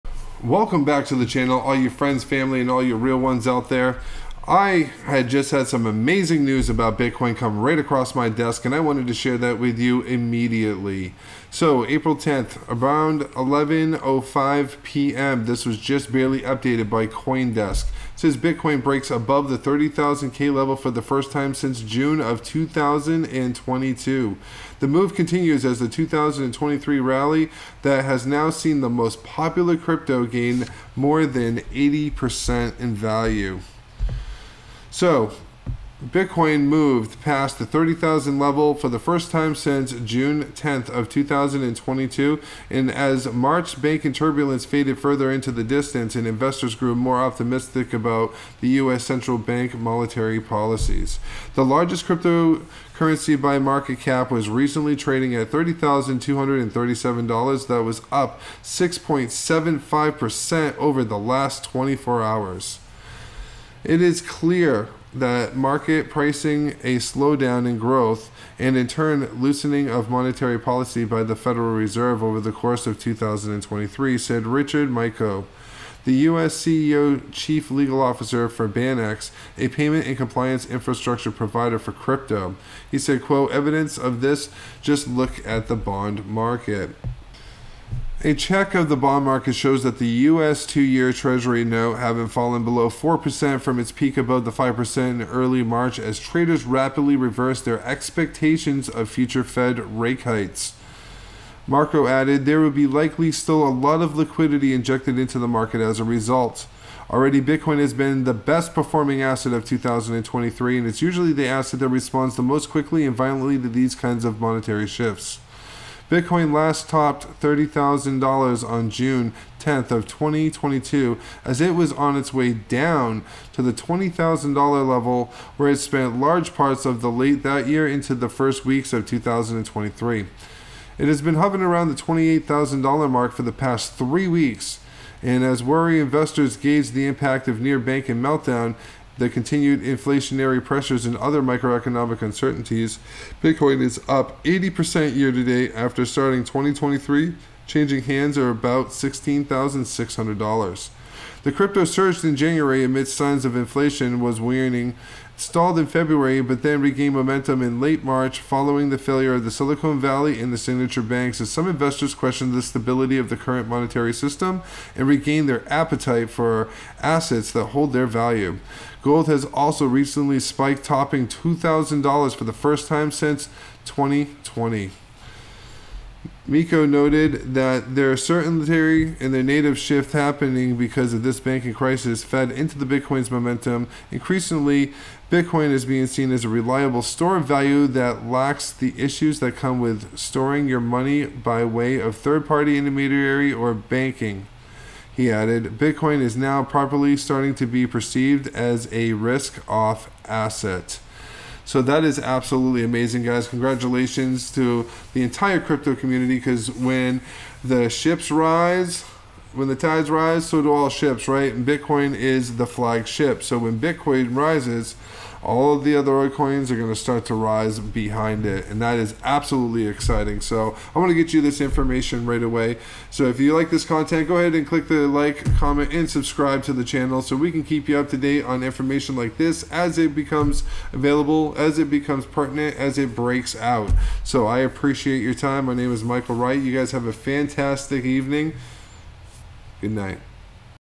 Bitcoin Breaking News!
In this breaking news video, we bring you the latest updates on the world's most popular cryptocurrency, Bitcoin. Bitcoin has broken the $30,000 mark for the first time since June 10, 2022, and we have all the details.

Our team of experts will analyze the market trends and discuss what this means for the future of Bitcoin and the wider cryptocurrency market. We will also explore the factors that have contributed to this significant rise in Bitcoin's value.

This video is a must-watch for anyone interested in cryptocurrency trading, investing, or simply keeping up with the latest news in the industry. So sit back, relax, and join us as we dive deep into the world of Bitcoin and its groundbreaking rise above the $30,000 mark.

Michael Wright
Global CEO
OnlineBusiness.App